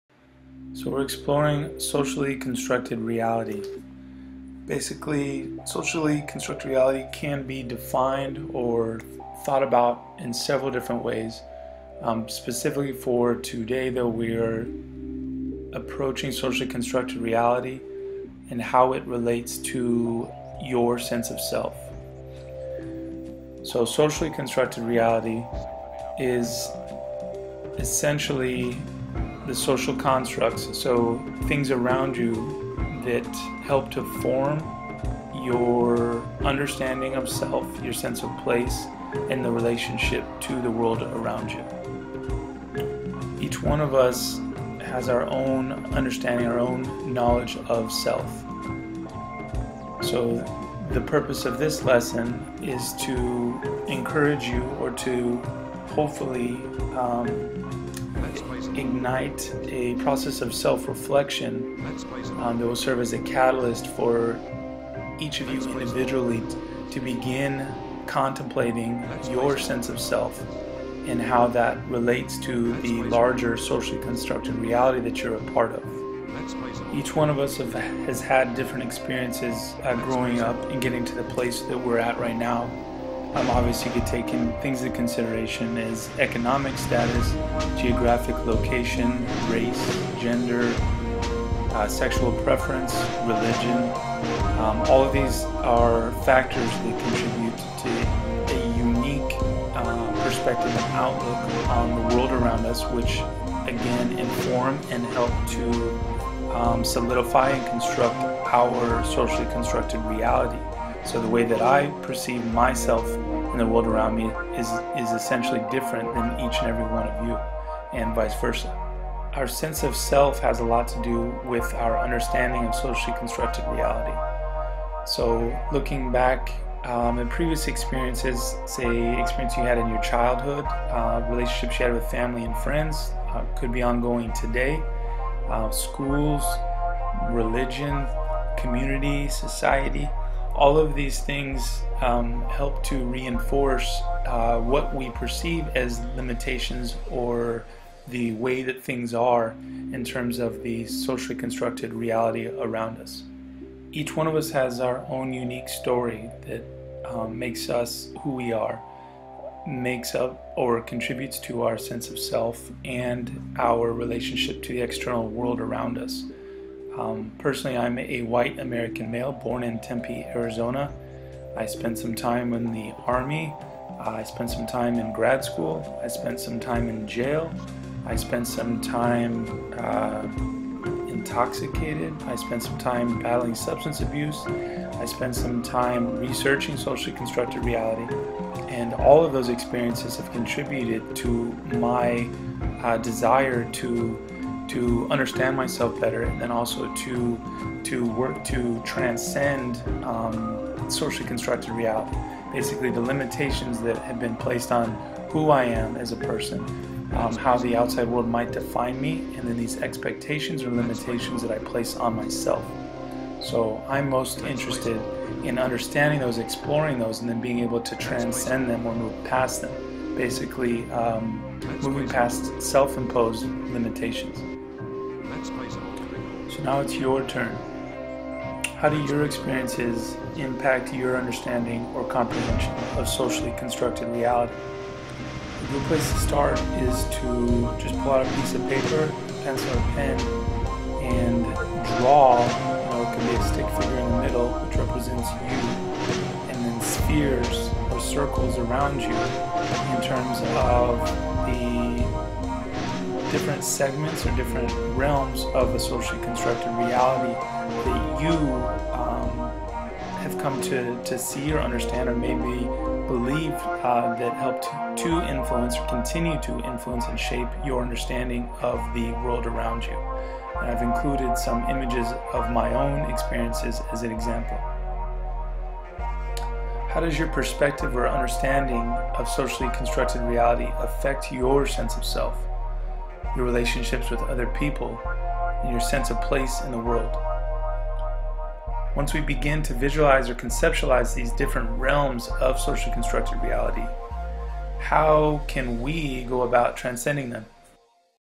Socially Constructed Reality & the Self
- brief presentation for Dr. Susan Rahman's - Intro to Sociology Course (College of Marin)